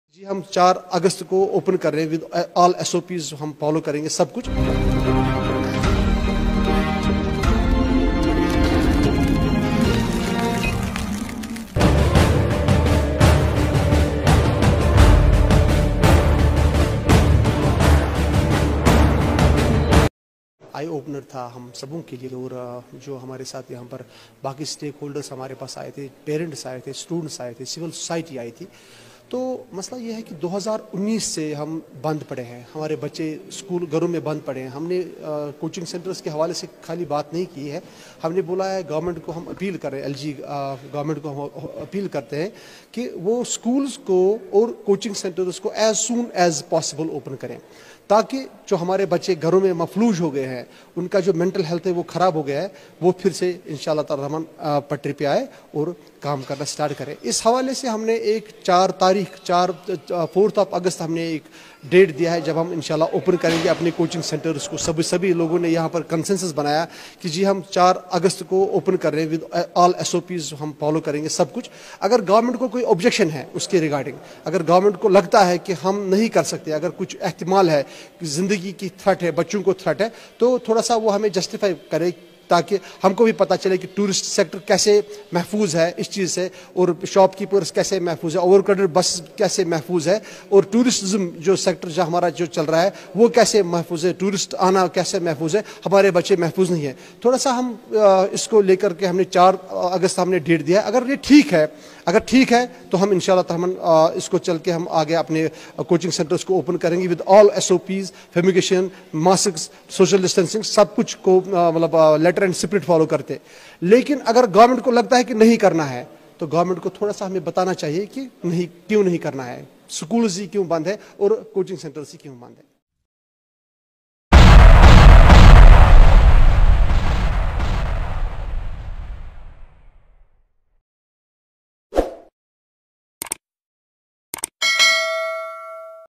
Parrypora Mission E Coaching Centre All set to Open from 4th August 2021 |Parrypora lockdown
After Meeting with Students, civil Society & Parents The Parrypora Coaching Administration has unanimously decided to Open Parrypora Coaching centres from 4Th August 2021